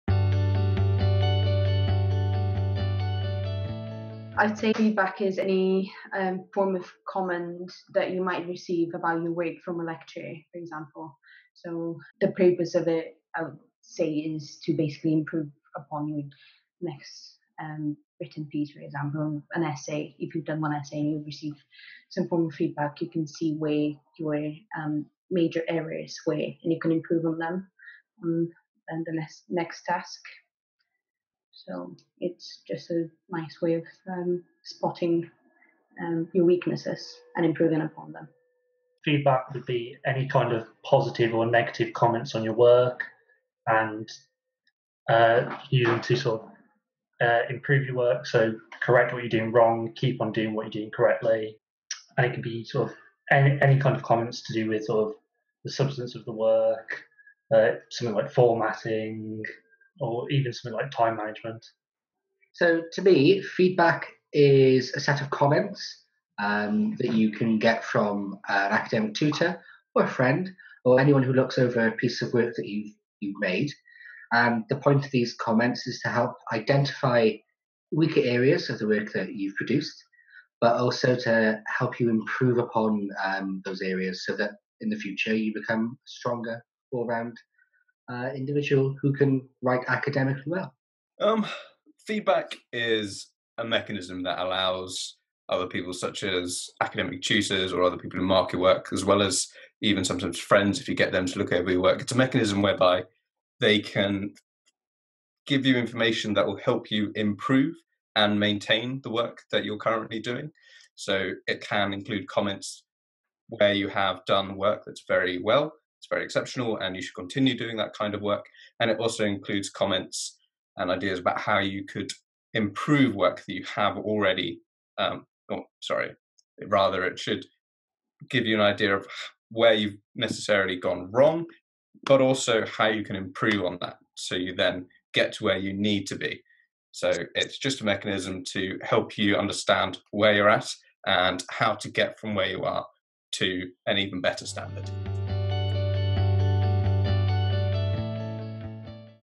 What is feedback and what is its purpose?
Four graduates discuss what they saw as feedback during their studies and what they felt the purpose of feedback was.